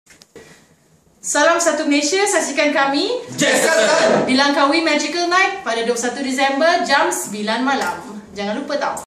Jesselton - Greeting Message!
Jesselton will be performing LIVE on the 21st December 2013 only at Langkawi Naturally Magikal 2013 Festival!! CATCH THEM LIVE!!